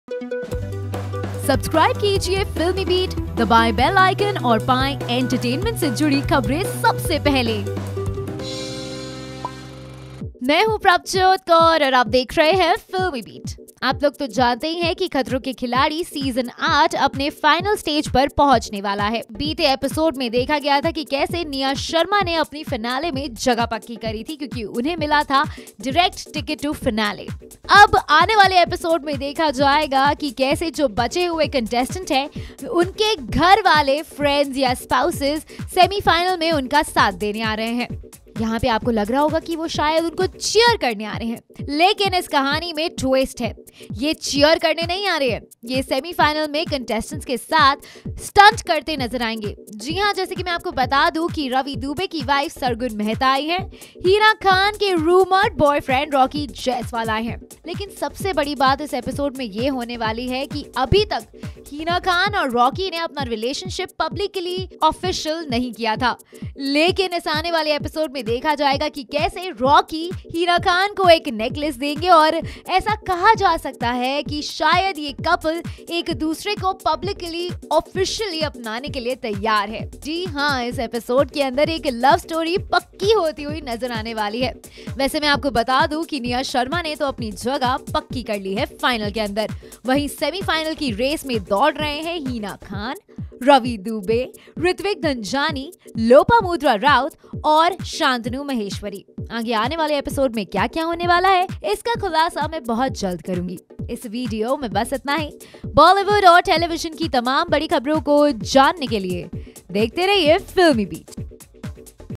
Khatron Ke Khiladi 8: Hina Khan and Rocky to make RELATIONSHIP OFFICIAL on Show | FilmiBeat
Khatron Ke Khiladi have reached to its final stage now. One of the toughest contender Hina Khan and her rumoured boyfriend will be seen in the coming episode where they will make their relationship official. Watch video to know more.